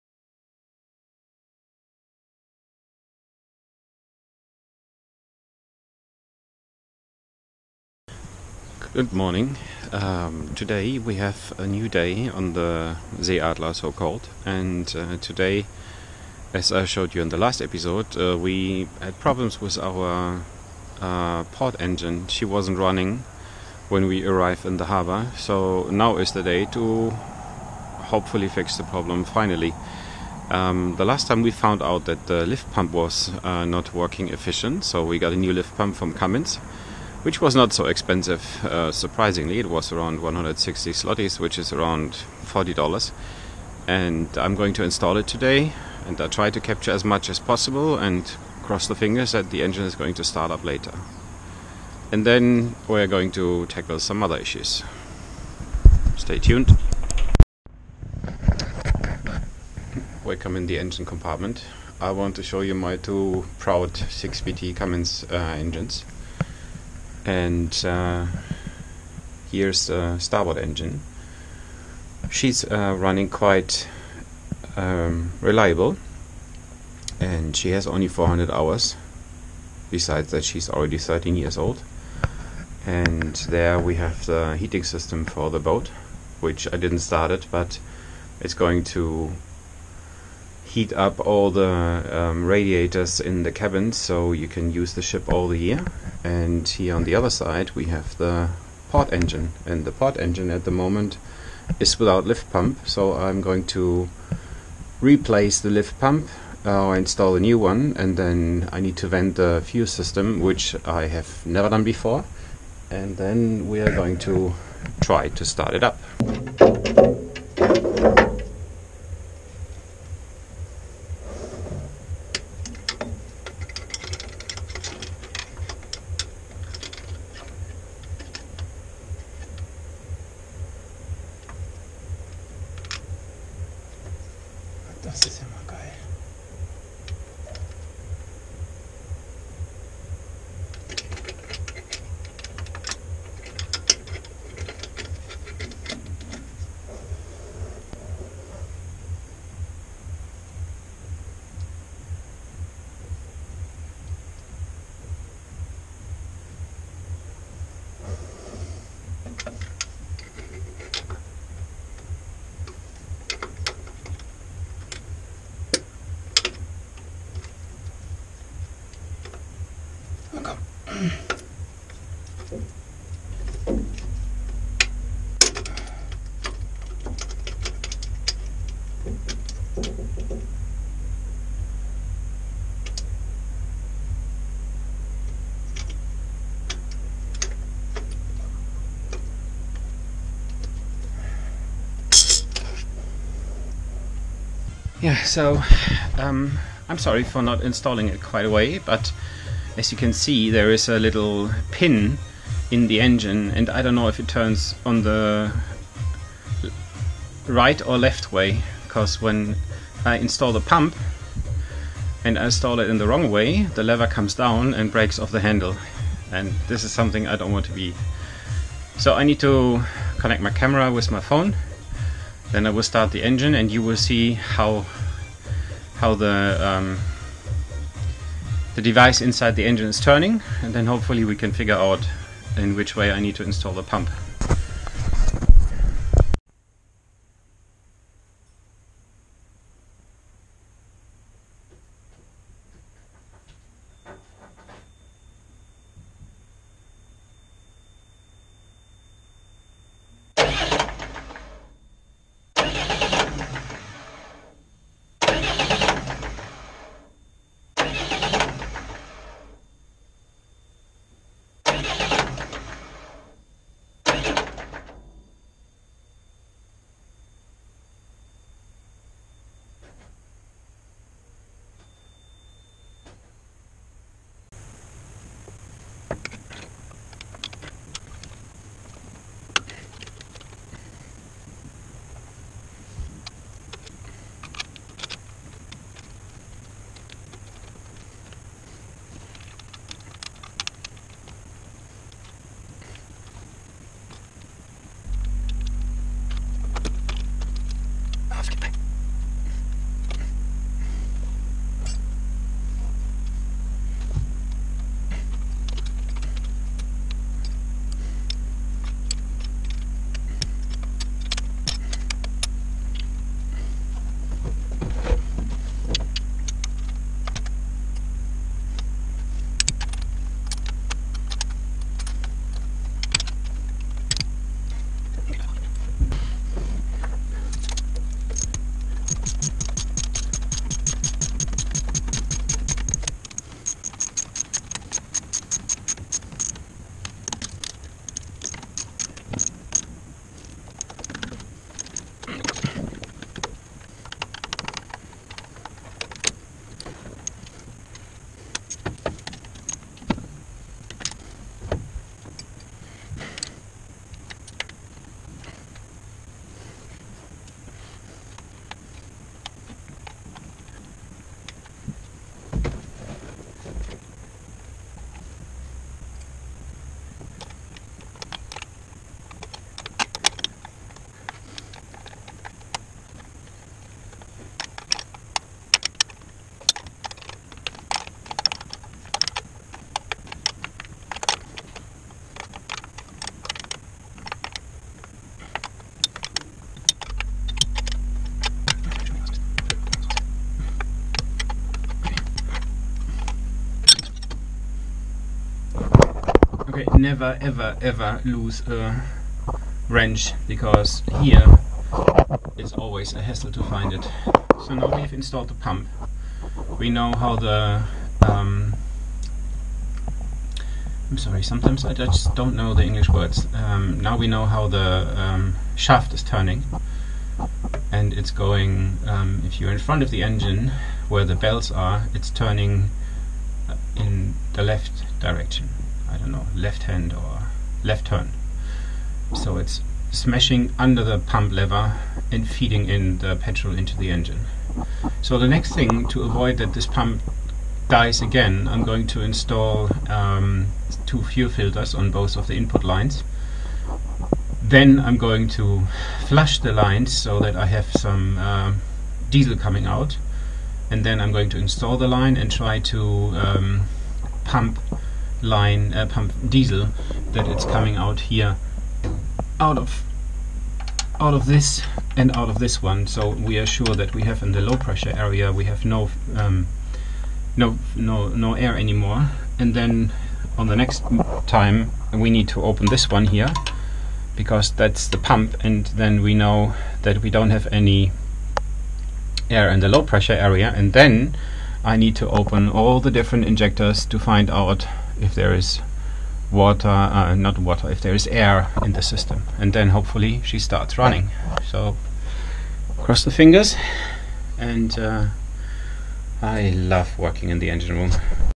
How to replace a Liftpump on Cummins 6BT 5.9 marine engine
In this video you will see how i exchange a liftpump on a Cummins 6BT 5.9 Engine. The liftpump stopped working because the membrane gave up. First i thought the filter was cloaked with rust particles from the steel tank, but after changing the filter i got aware that the engine still does not charge. There will be more videos of how tos as i have to install fuel filters and also how to vent the fuel system on these engines.